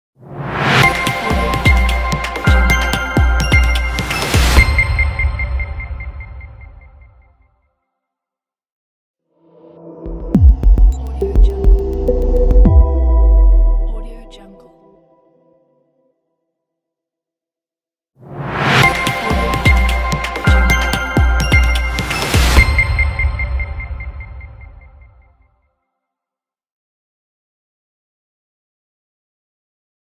Digital Motion Logo Reveal (After Effects template)
Digital Motion Logo Reveal Create new technologic and modern product with this After Effects Template. The project includes a video tutorials, so that even a novice will figure out how to customize it. You can use any logo with alpha channel (.png .ai .eps etc).
4K 3840×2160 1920×080 Full HD, 1080p composition for rendering is included
Support 24/7 with pleasure